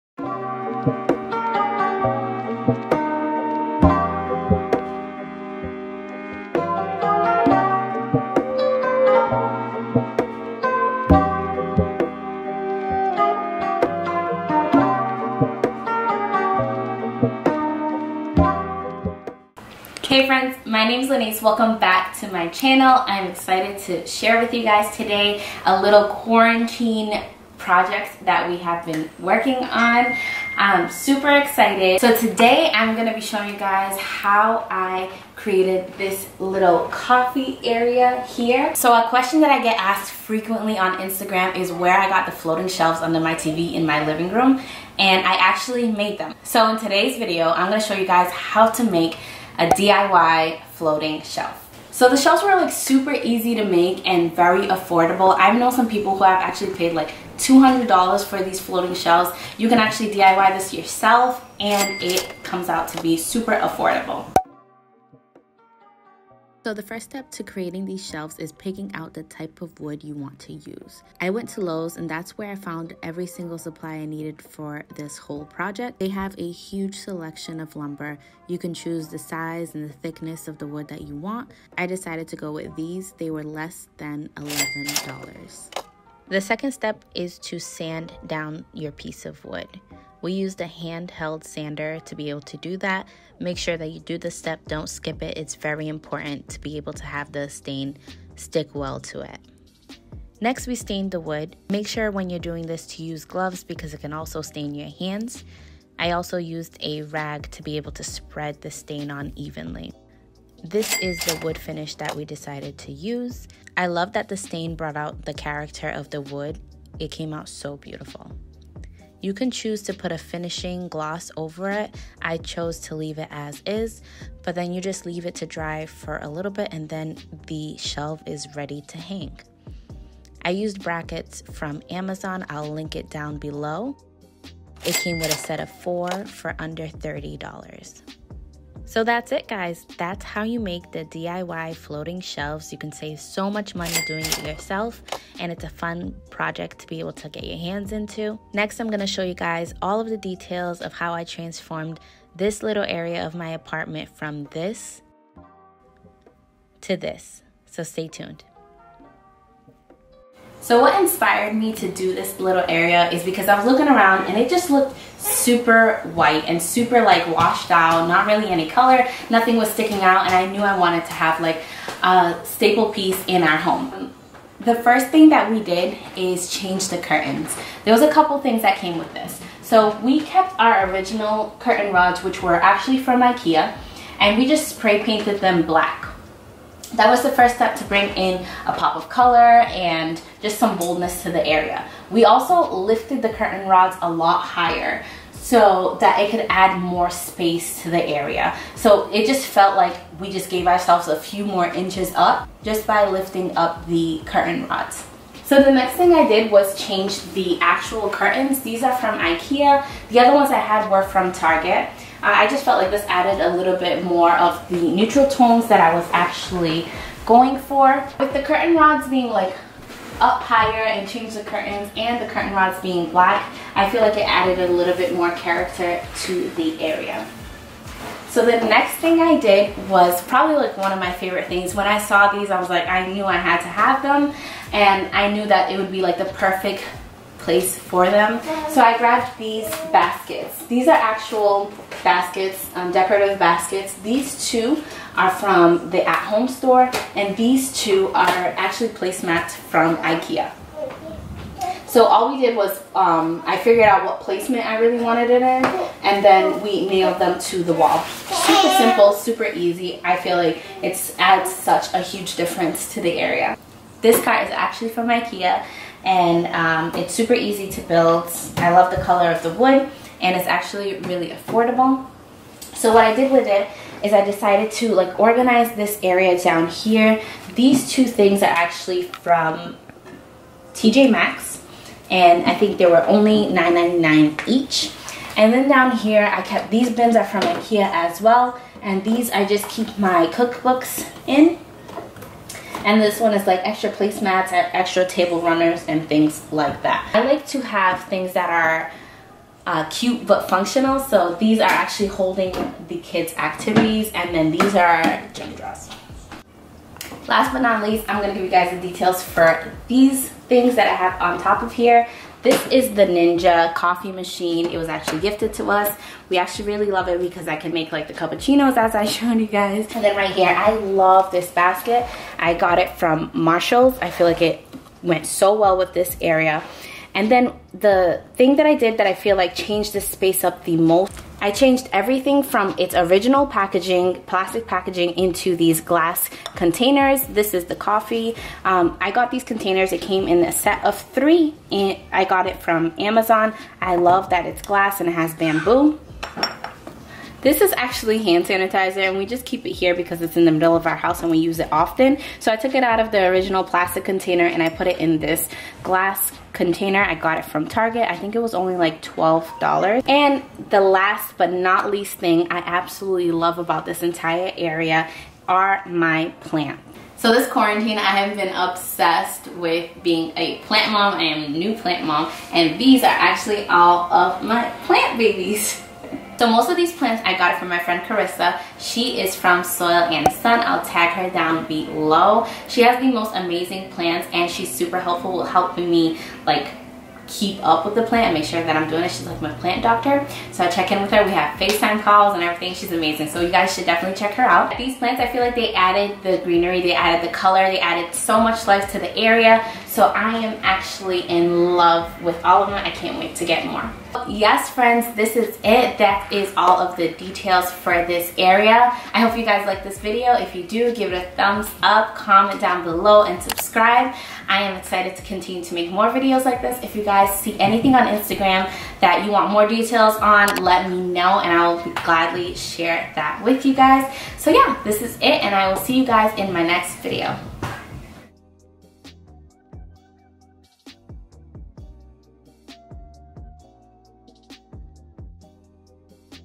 DIY Floating Shelf for Less than $50! || Boho Coffee Area
Home decor is one of my favorite things! Added a more boho vibe to this small coffee area in my dining room. Floating shelves can go to up towards $200, here's how to make it for less than $50!

Curtain Rod
Curtain: Ikea (out of stock!)
Coffee Machine
Glass Pump Bottle
Glass containers with Bamboo Top
Wall Baskets: Ikea, At Home Store
White Basket with Bamboo top: TJ Maxx
White Storage Bins